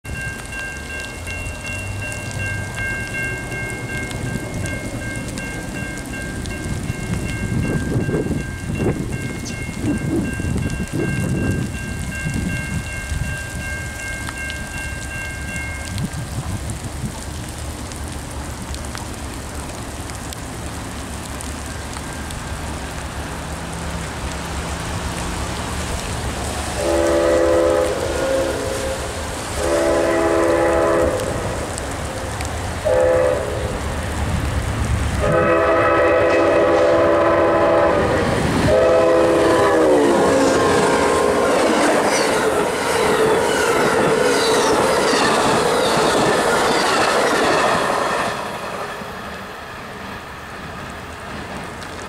Amtrak 20 flying north through Thomasville, NC w/ Phase III Viewliner dinner (11/24/2018)
Amtrak 20 flying north through Thomasville, NC w/ Phase III Viewliner dinner.